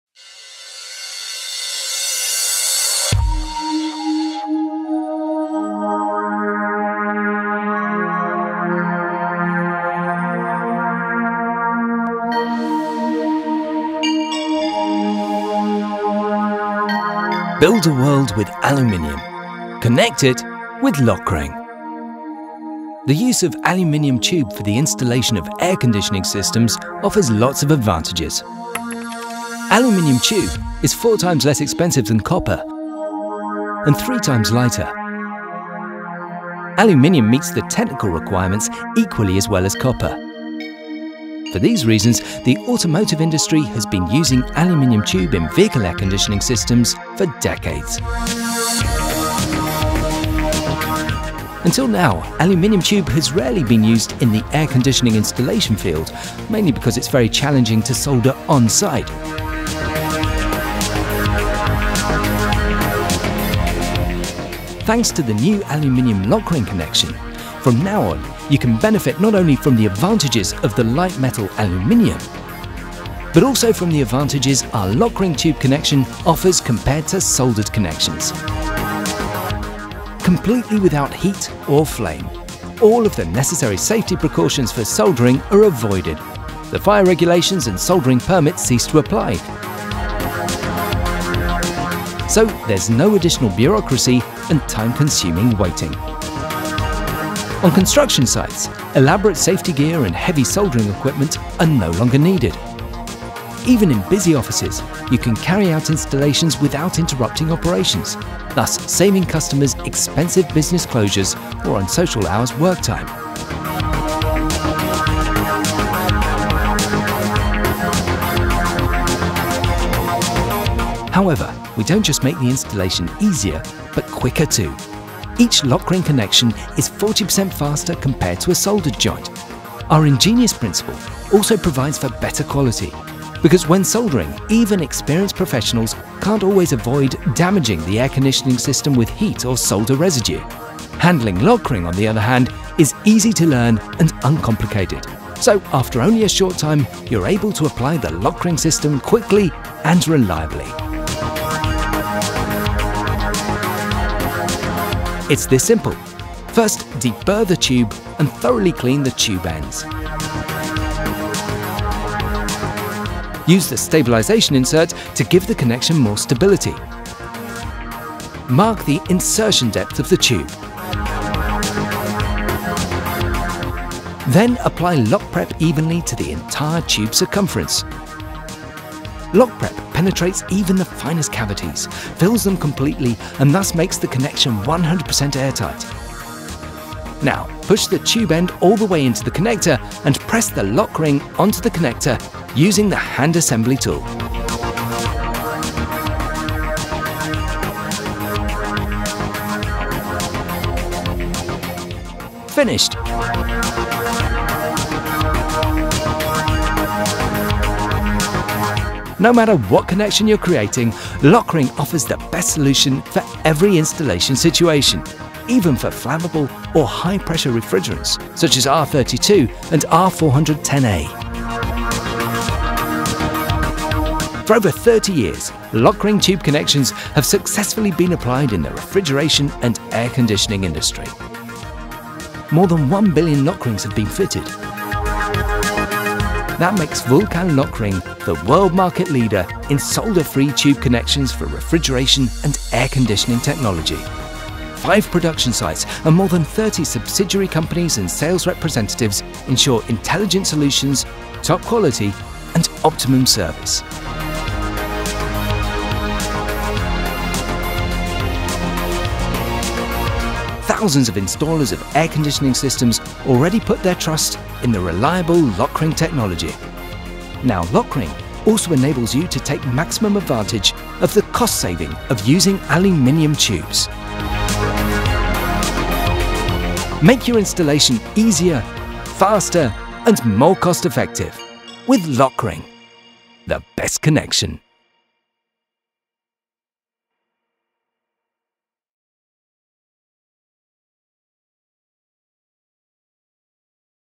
Solder Free ALUMINUM Refrigeration and Air Conditioning piping - مواسير تكييف من الألمنيوم بدون لحام
aluminum piping in air-conditioning !! why not
with lokring innovative solder free pipe connectors, now it is time for aluminum to replace copper piping. no need to worry about the difficulty of aluminum braze any more, we will connect joints with lokring
ALUMINUM is less expensive than copper
ALUMINUM is lighter than copper

AFIFI is proud to be the only distributor of LOKRING products in our region